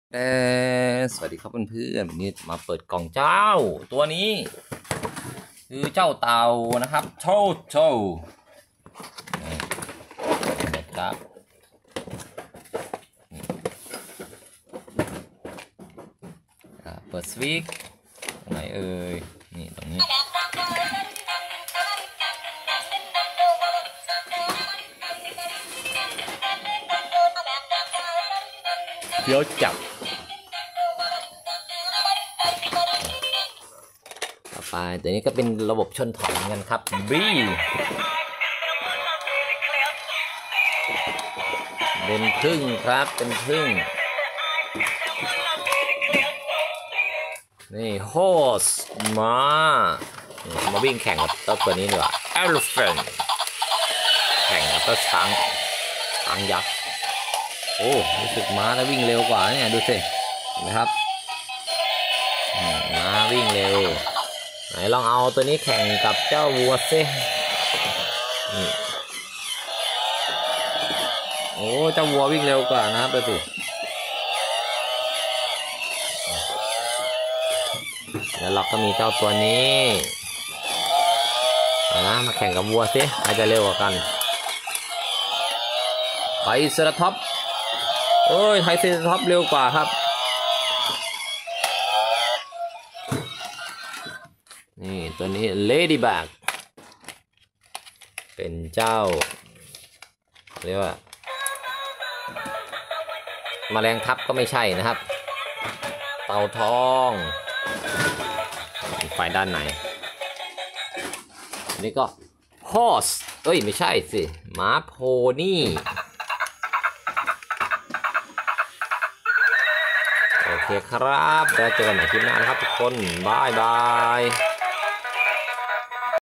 เปิดกล่องของเล่นเต่าใส่ถ่าน มีเสียงมีไฟ ชนถอยเดิน มากับม้า วัว จระเข้ ผึ้ง ลิง ไดโนเสาร์
เปิดกล่องของเล่นเต่าใส่ถ่าน มีเสียงมีไฟ ชนถอยเดิน มากับม้า วัว จระเข้ ผึ้ง ลิง ไดโนเสาร์ dza999 turtle horse @dza9999